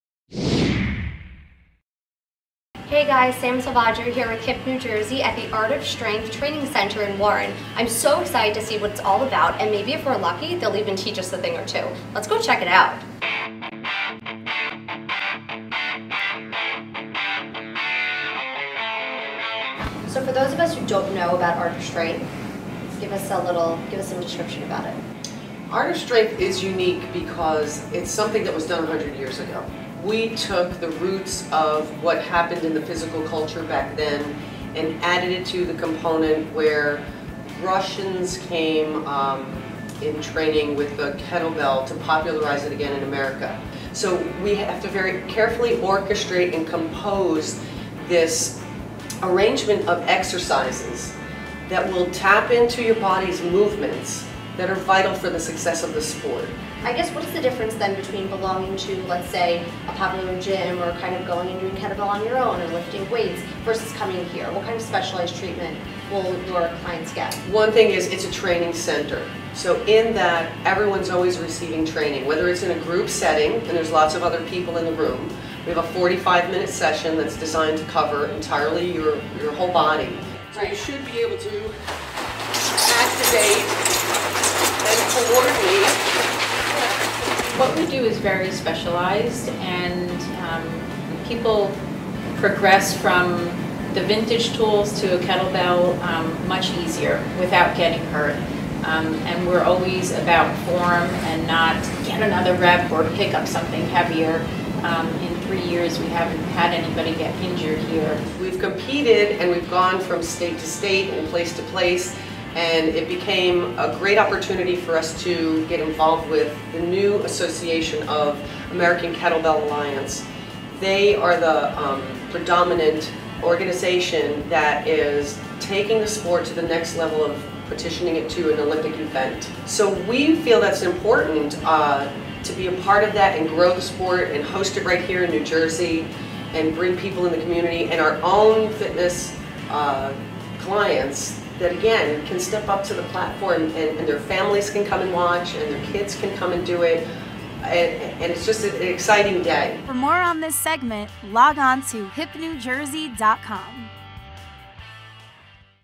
Hip New Jersey: Art of Strength
Art of Strength in Warren is helping HipNJ citizens become “Jersey Strong(er)!”

HipNJ’s Samantha Selvaggio paid a visit to this unique fitness facility, and worked out with co-owners Valerie Pawlowski and Lisa Fleischhacker.

At Art of Strength, you can expect personalized, one-on-one service. Trainers don’t rely on using machines and benches to sculpt your body; they use natural body movements to help build from within, which greatly cuts down on injury.  Methods include kettlebells (a cast-iron or steel weight resembling a cannonball with a handle), “Ropes Gone Wild” (a high-intensity, zero-impact conditioning technique using unhooked climbing ropes) and suspension, an extension of traditional body weight exercises.

“We took the roots of physical culture from 100 years ago and added it to the components of today,” explained Valerie.

Art of Strength’s doors are open to “every body.”  Men, women and children are all welcome, and are treated with great respect.  Teachers enrich not only the physical aspects of training, but also strive to improve the mental and emotional well-being of students.

For more on Art of Strength, please visit their website.